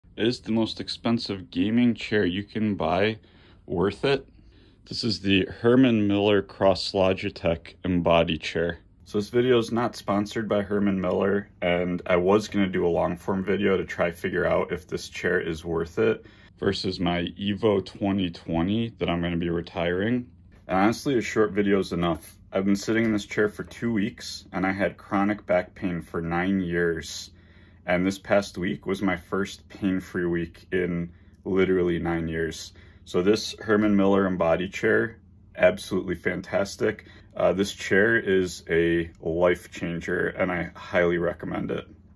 Herman Miller Embody Gaming Chair Quick Review!
The Herman Miller is worth every penny from my perspective. It has fixed 9 years of chronic back pain I had been suffering from.